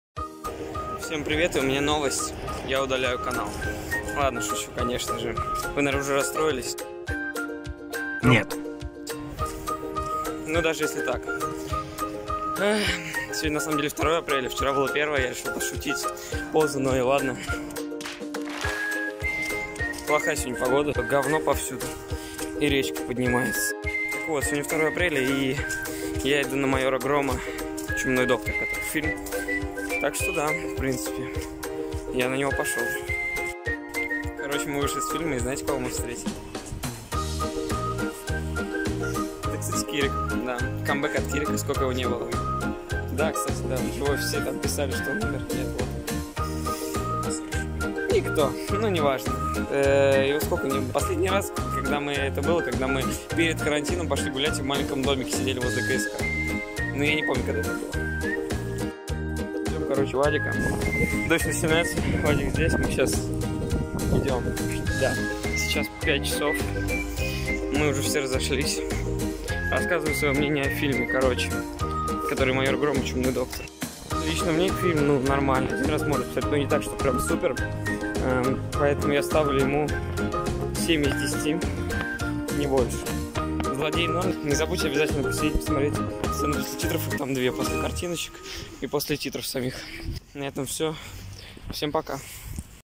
Vlog №69 Апрельская весна/Мнение о фильме "Майор Гром:Чумной Доктор"/Comeback от Кирика.
В этом видео апрельская весна из-за которой поднимается речка,идём на фильм "Майор Гром:Чумной Доктор",встречаем Кирика и гуляем и рассказываю своё мнение по поводу фильма.